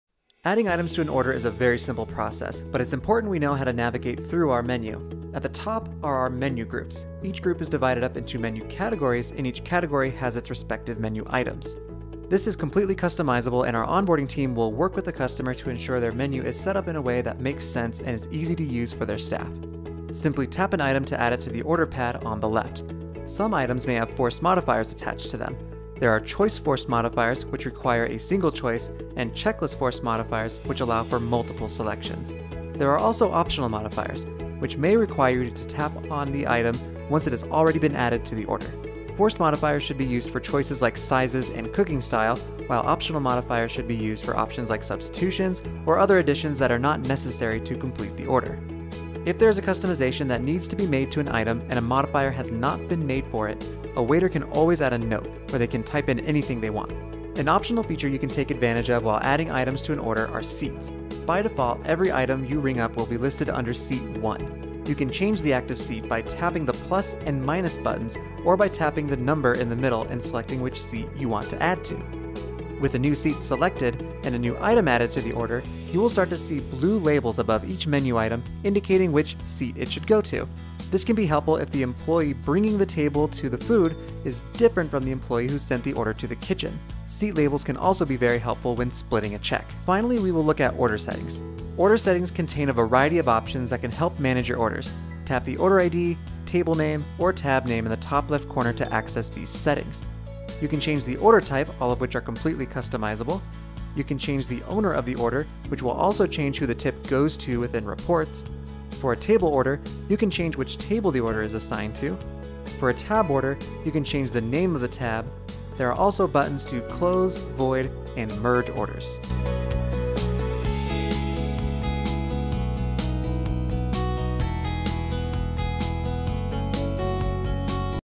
Menu: Items, Modifiers, Notes, Seats, Order Settings
Adding items to an order is a very simple process, but it’s important we know how to navigate through our menu. At the top are our menu groups. Each group is divided up into menu categories, and each category will have its respective menu items.

This is completely customizable, and our onboarding team will work with the customer to ensure their menu is set up in a way that makes sense and is easy to use for their staff.

Tap an item to add it to the order pad on the left.

Some items may have forced modifiers attached to them. There are choice forced modifiers, which require a single choice, and checklist forced modifiers, which allow for multiple selections.

There are also optional modifiers, which may require you to tap on the item once it is already added to the order.

Forced modifiers should be used for choices like sizes and cooking style, while optional modifiers should be used for options like substitutions, or other additions that are not necessary to complete the order.

If there is a customization that needs to be made to an item, and a modifier has not been made for it, a waiter can always add a note, where they can type in anything they want.

An optional feature you can take advantage of while adding items to an order are seats. By default, every item you ring up will be listed under Seat 1. You can change the active seat by tapping the plus and minus buttons, or by tapping the number in the middle, and selecting which seat you want to add to.

With a new seat selected, and a new item added to the order, you will start to see blue labels above each menu item indicating which seat it should go to. This can be helpful if the employee bringing the table their food is different from the employee who sent the order to the kitchen.

Seat labels can also be very helpful when splitting a check.

Finally, we will look at order settings. Order Settings contain a variety of options that can help manage your orders. Tap the order id, table name, or tab name in the top left corner to access these settings.

You can change the Order Type, all of which are completely customizable.

You can change the owner of the order, which will also change who the tip goes to in reports.

For a table order, you can change which table the order is assigned to.

For a tab order, you can change the name of the tab.

There are also buttons to close, void, and merge orders.